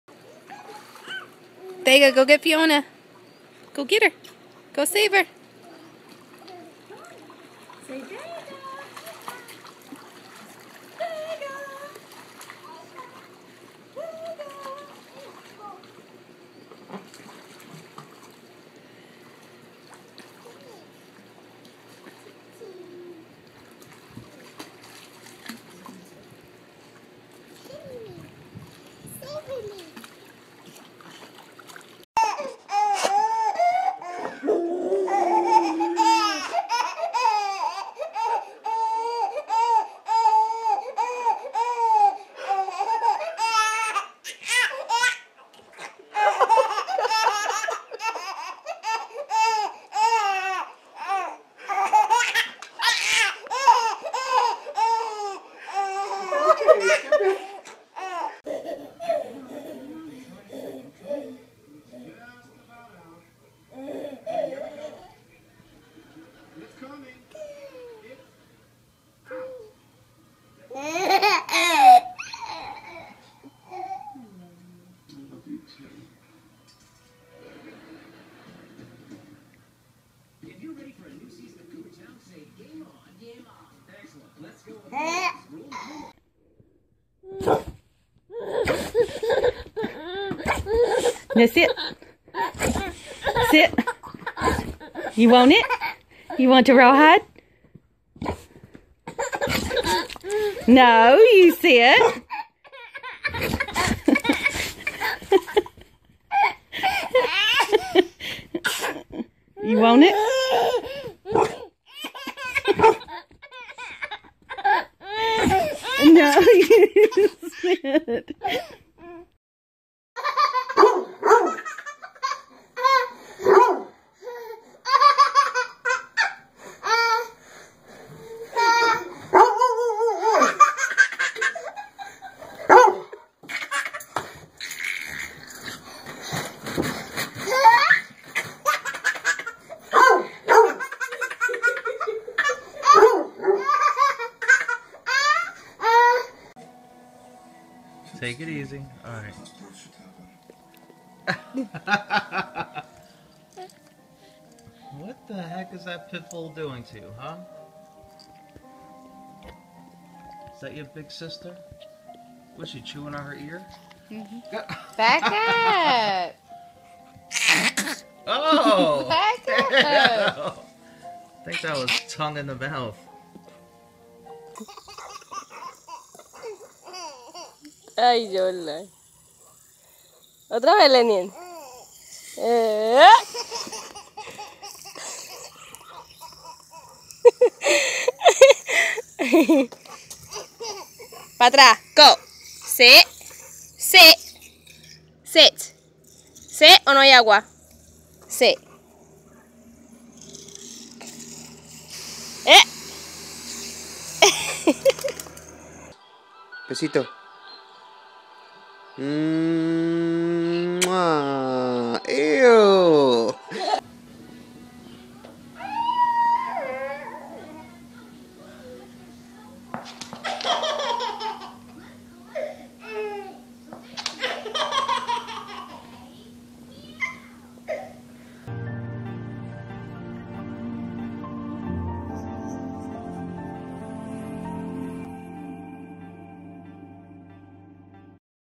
Pit Bull Protects Baby Compilation
Pit bulls were once known as the "nanny dog" because they were loyal and protective when it came to staying with baby.